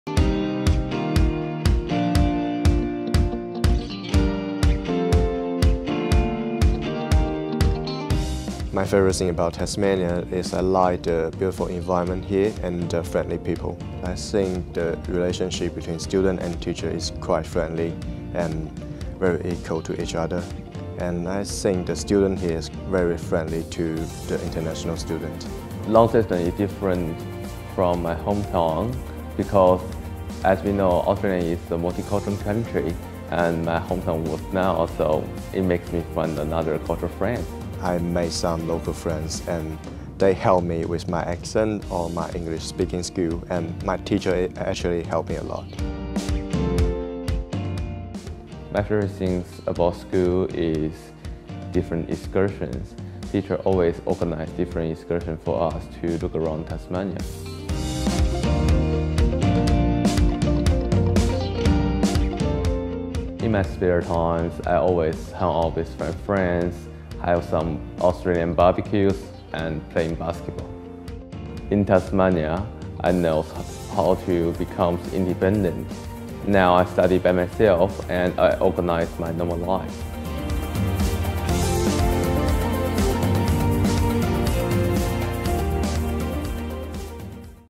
Tasmanian Government Schools- short video Chinese simplified
A short video on why Tasmanian Government Schools are an excellent choice for International Students. Video with Chinese Simplified Subtitles.